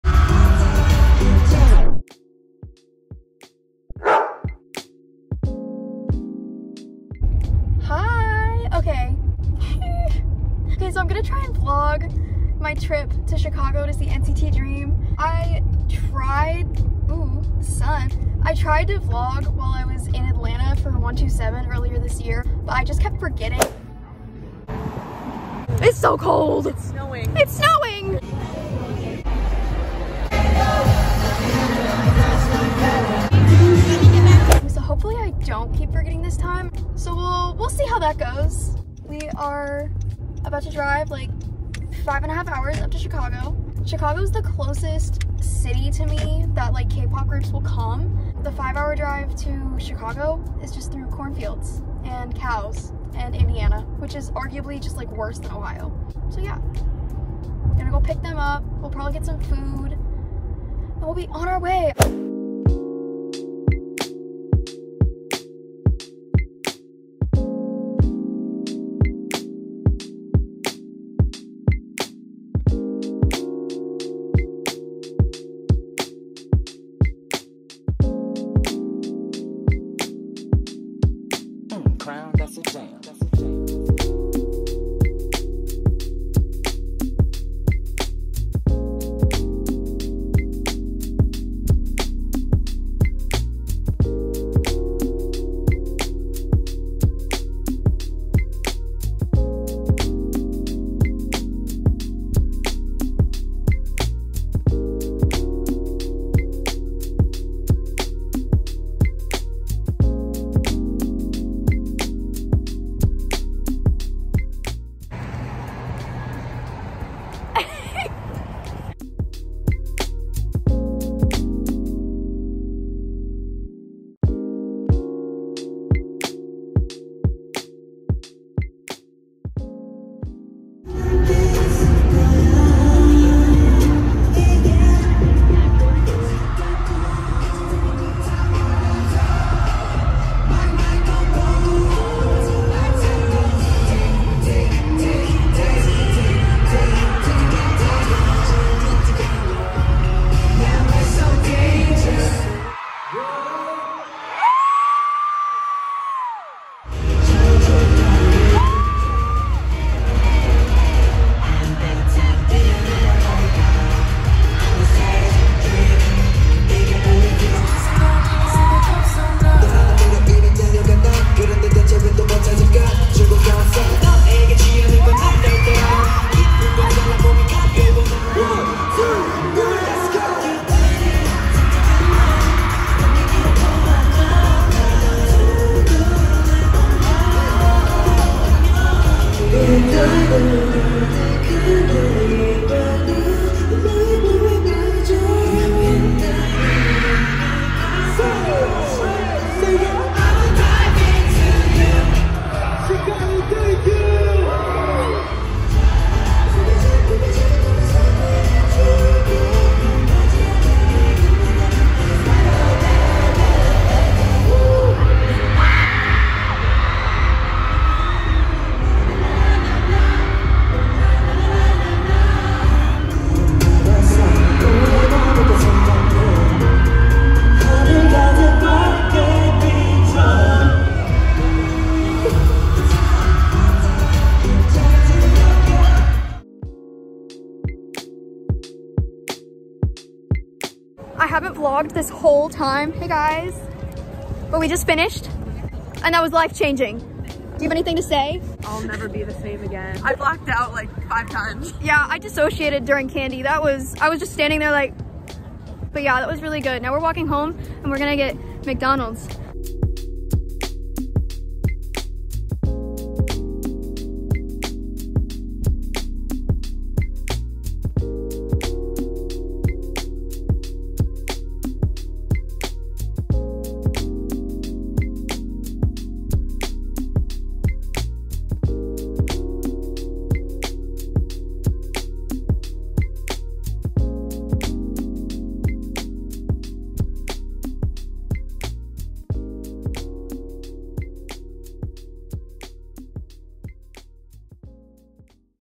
nct dream in chicago! | VLOG apr.2023
this is three months late but ITS HERE. i actually had so much fun putting this together why did it take me so long ummm LOL

nct dream put on such a fun show! i miss this weekend so much